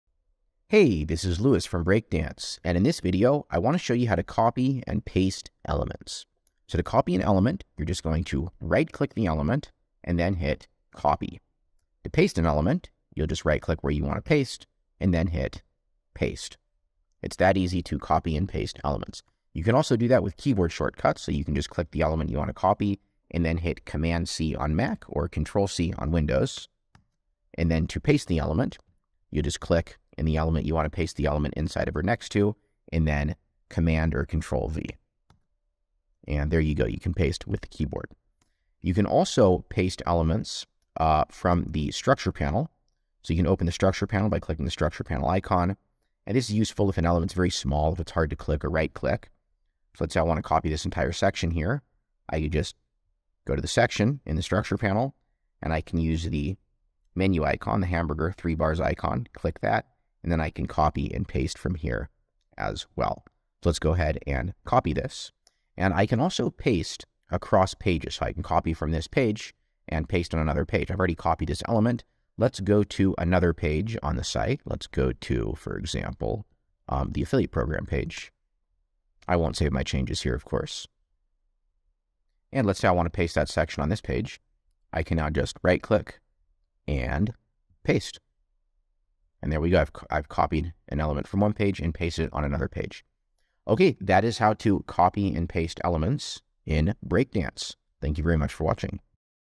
How To Copy & Paste Elements In Breakdance
Breakdance’s convenient keyboard shortcuts make it easy to copy & paste elements.

In this video Louis from Breakdance will show you how to copy & paste elements using the right-click menu, keyboard shortcuts, or the Structure panel.

Join thousands of other enthusiastic Breakdance users in our Facebook group